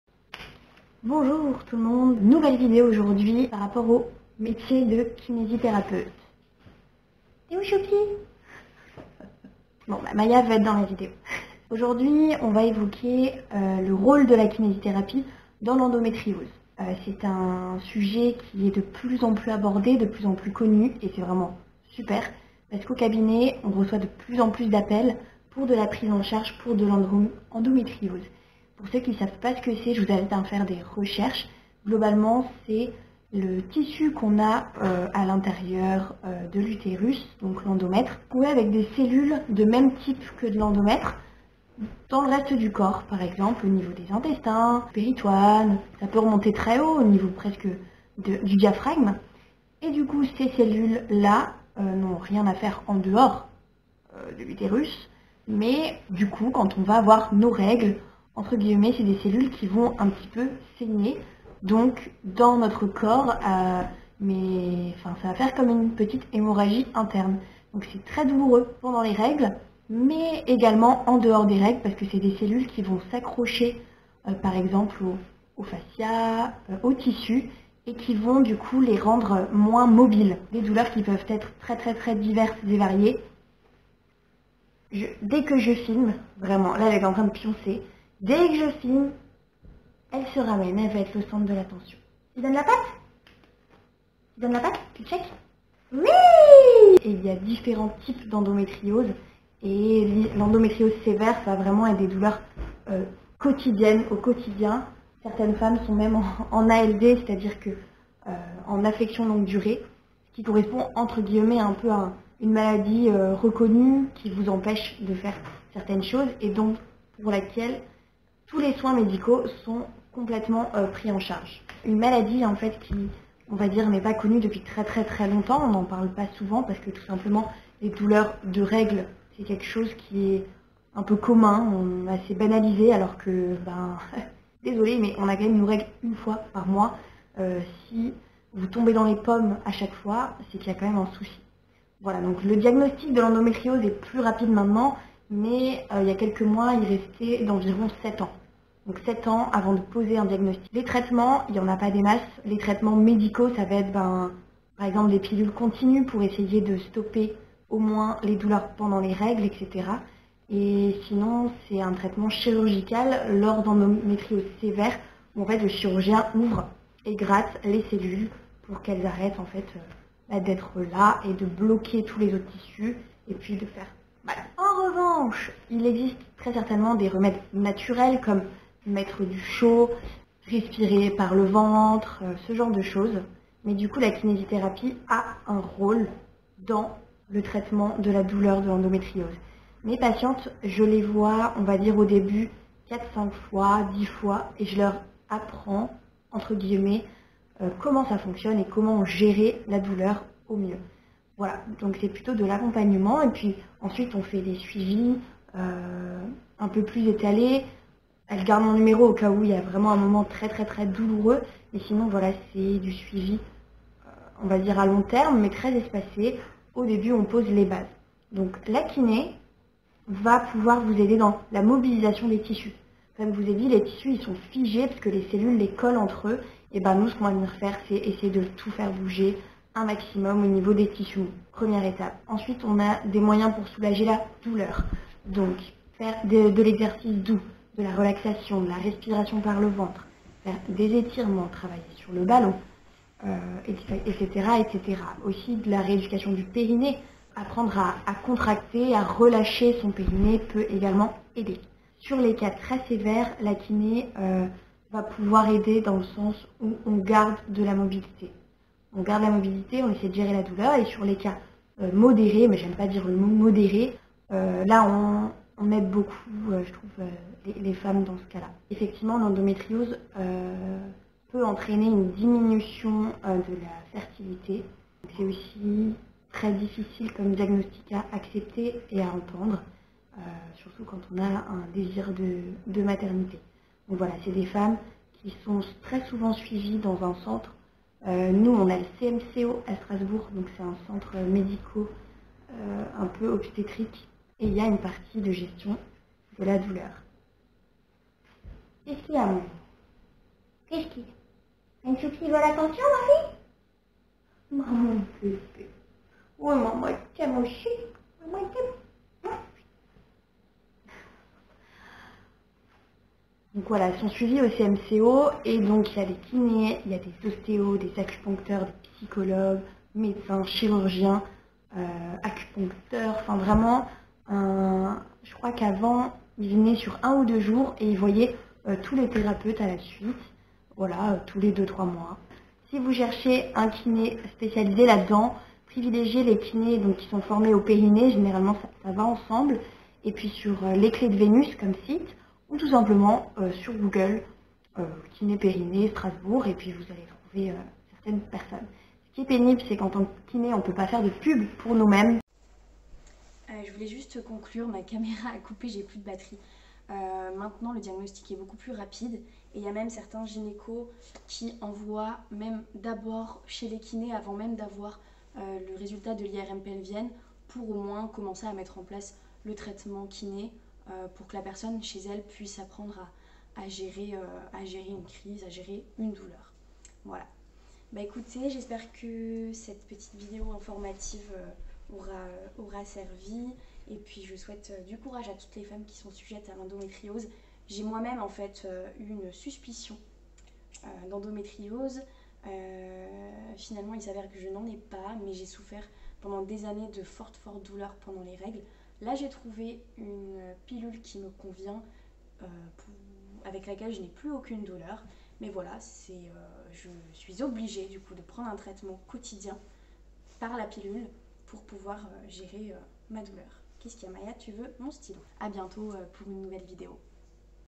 KINÉ et ENDOMÉTRIOSE
La gestion de la douleur de l'endométriose relève bien des champs de compétence d'un kiné formée en sphère uro-gynéco :)
N'hésitez pas à consulter!